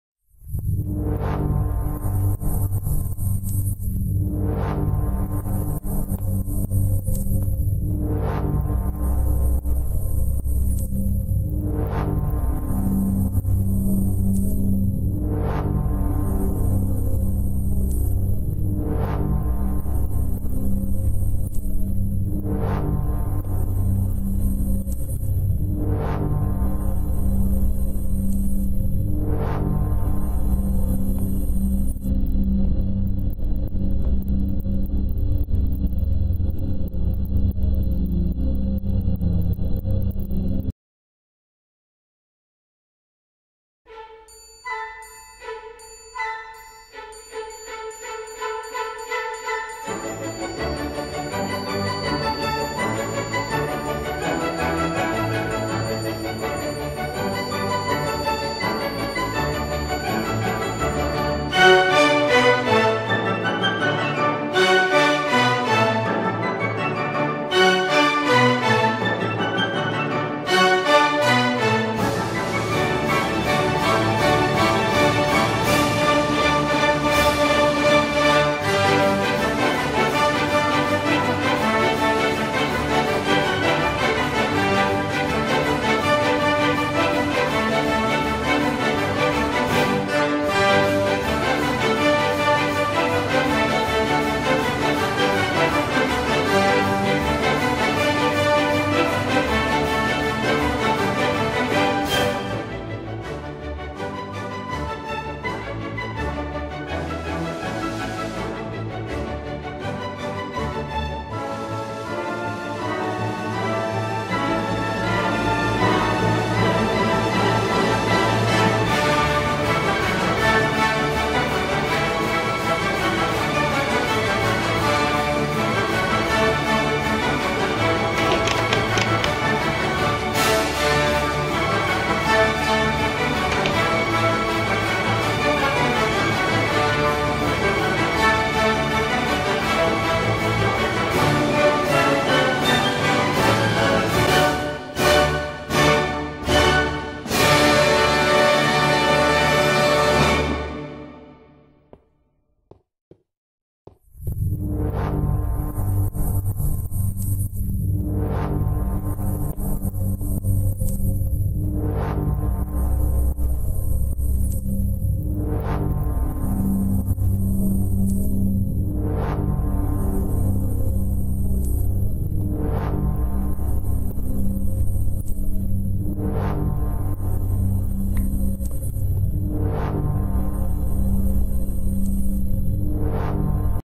Mike's Footage - #3 Clone Frenzy
I found out I could clone myself an unlimited amount of times, so I did. I'm close to reaching Doorjang HQ, I know it. Wish me luck.